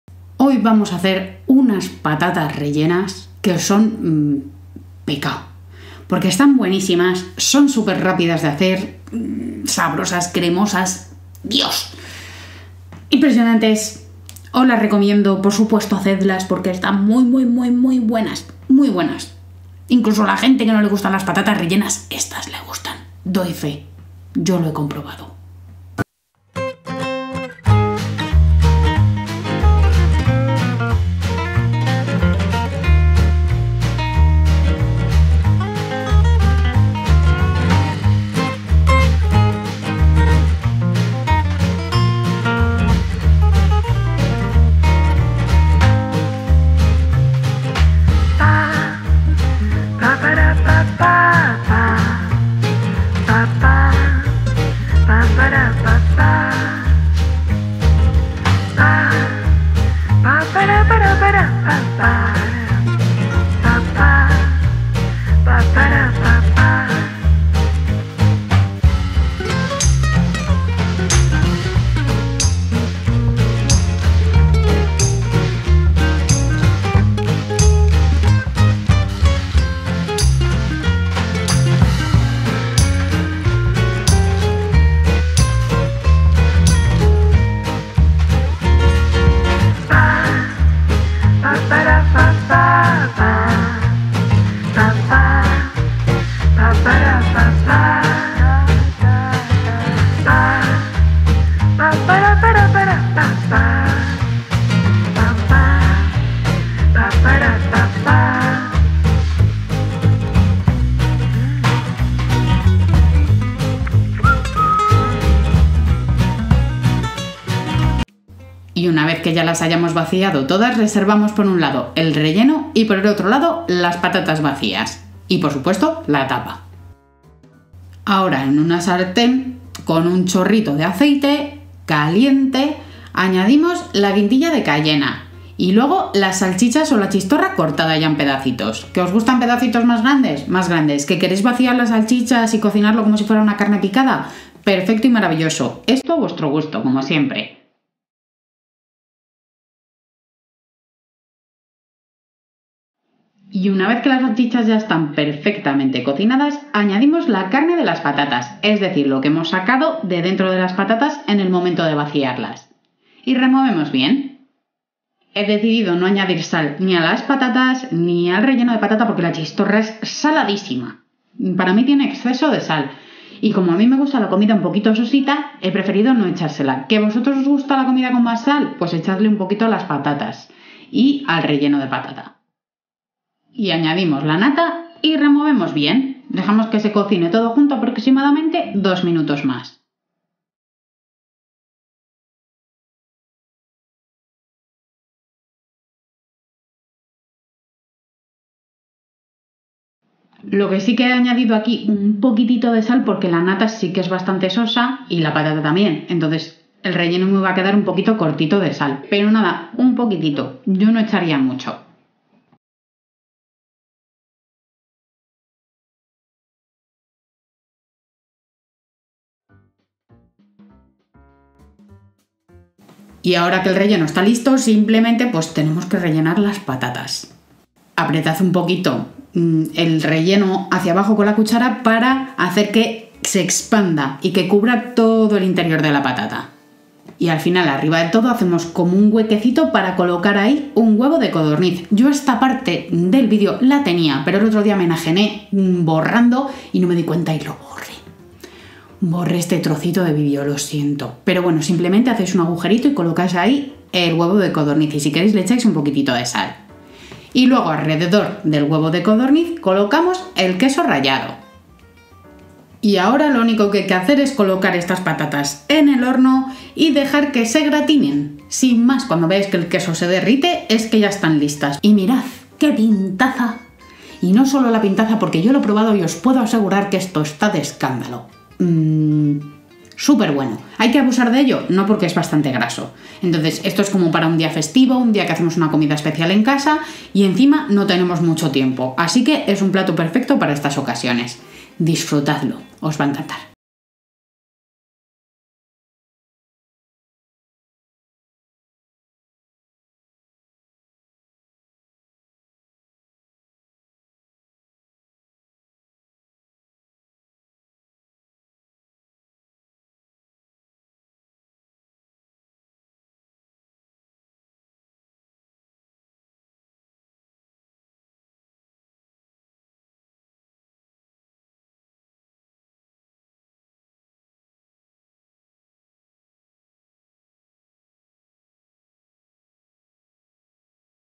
Cuando Pruebes El Relleno De Estas Patatas Te Vas A Enamorar...¡¡DELICIOSO!!
Cuando pruebes el relleno de estas patatas te vas a enamorar...¡¡Delicioso!! Y lo mejor de todo, extremadamente sencillo y rápido porque asaremos las patatas en el microondas. Una receta imprescindible en todo recetario que se precie.